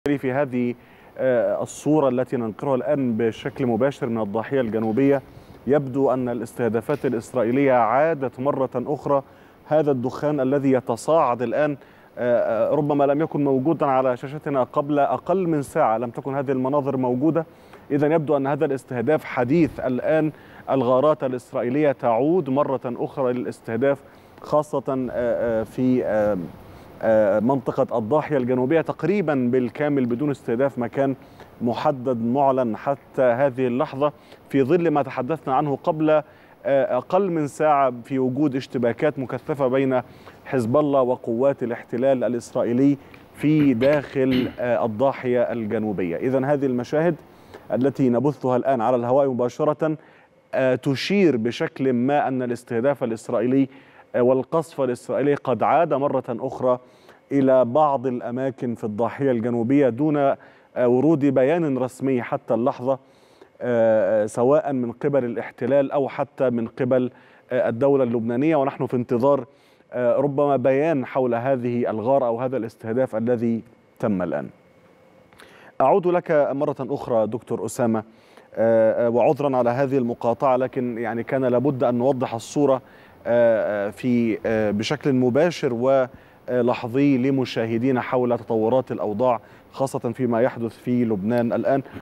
تصاعد أعمدة الدخان من الضاحية الجنوبية لبيروت
11/10/2024